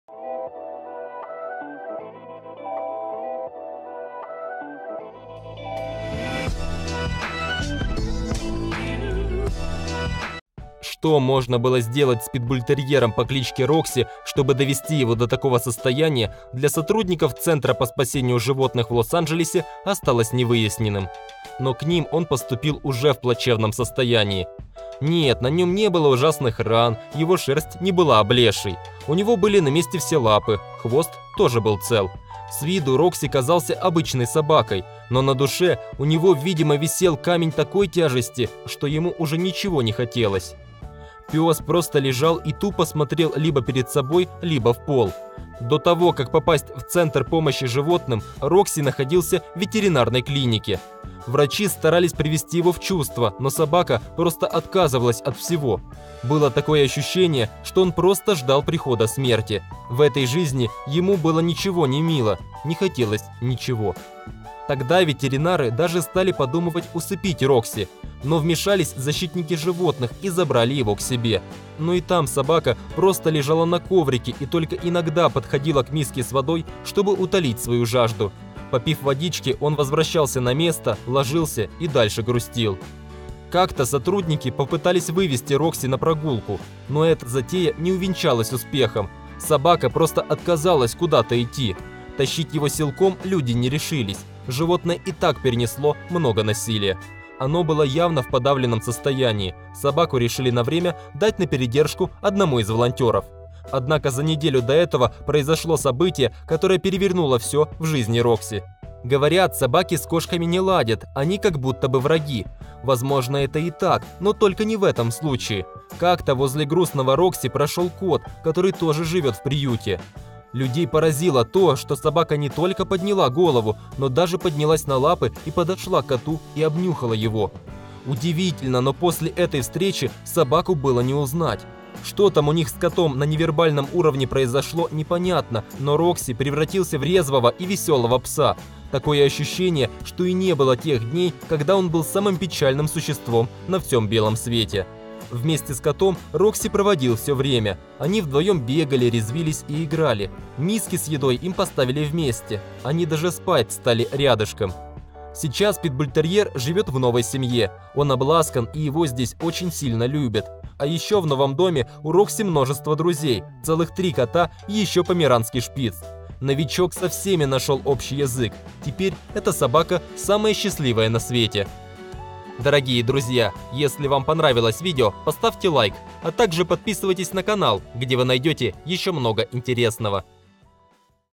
Пес лежал и ожидал врачей с последним уколом.
Что можно было сделать с питбультерьером по кличке Рокси, чтобы довести его до такого состояния, для сотрудников центра по спасению животных в Лос-Анджелесе осталось не выясненным. Но к ним он поступил уже в плачевном состоянии.